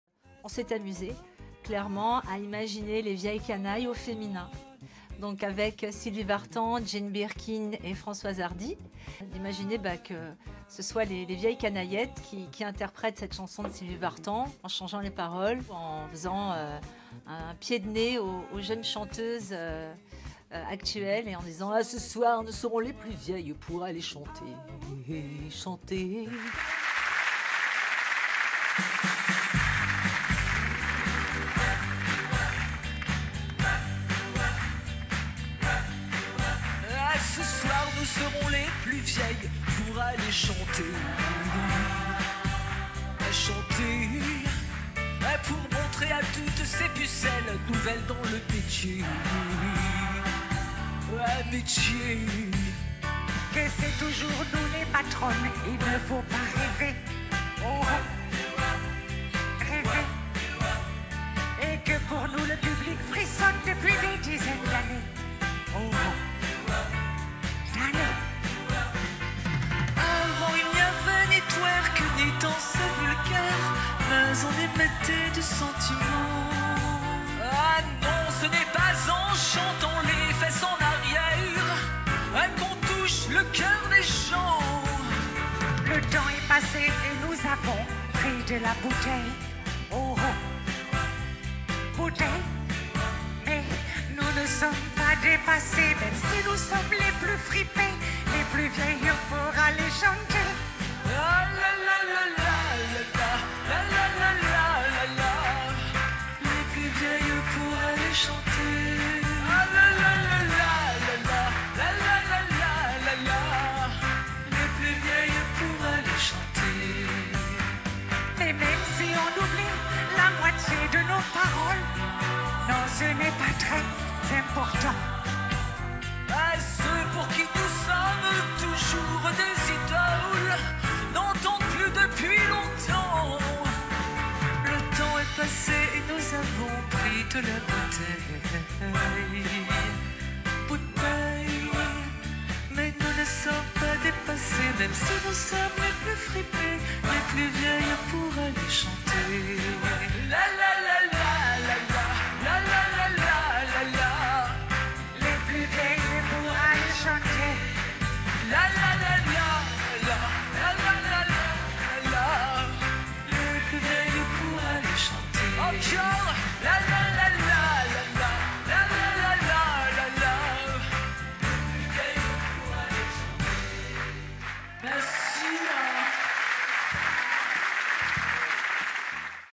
Les vieilles canail(lettes)
Hier soir sur France 3, j’ai eu la grande chance de pouvoir rassembler 3 dinosaures de la chanson française : Sylvie vartan, Françoise Hardy et Jane Birkin.

Après donc les vieilles canailles, voici donc les vieilles canaillettes qui nous expliquent qu’elles sont encore là avec un public bien fidèle ☺️